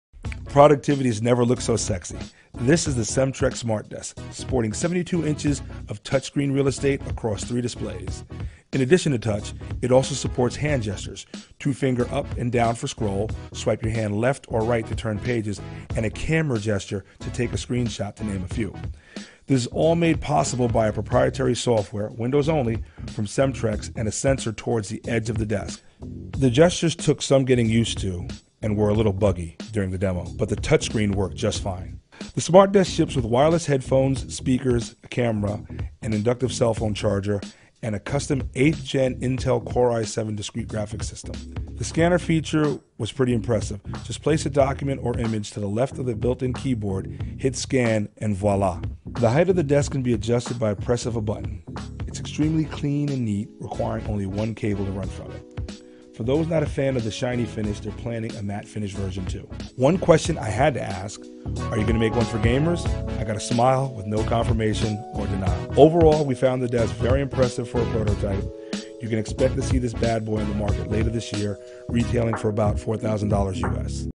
"미래에서 온 업무용 책상" 셈트랙스 스마트 데스크
증강현실 및 스마트 기술 연구 개발사인 셈트랙스(CENTREX)에서 공개한 '스마트 데스크' 는 24인치 터치스크린 모니터 3개를 탑재한 PC 올인원 형태의 업무용 데스크이다.

이 제품은 셈트랙스에서 개발한 소프트웨어 윈도우 센서를 탑재해 손동작만으로 화면을 확대하거나 넘기고 스크린샷을 남길 수도 있으며  책상 위에 문서를 올려놓는 것만으로 스캔을 할 수 있다. 또한 버튼 하나로 높낮이 조절이 가능하고 스마트폰 무선 충전 및 핸즈프리 통화 기능도 지원한다.